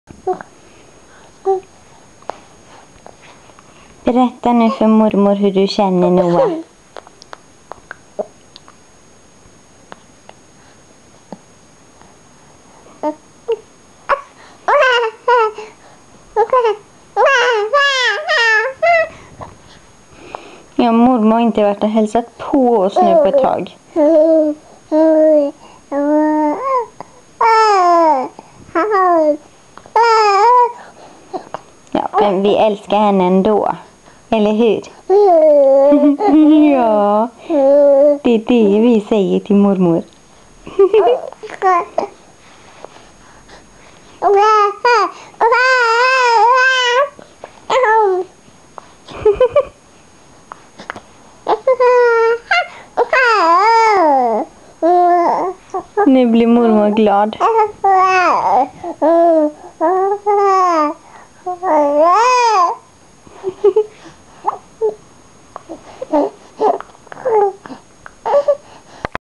Noahs hälsning till mormor
Noah skäller på sin mormor som inte hade hälsat på honom på ett tag, han saknade henne =).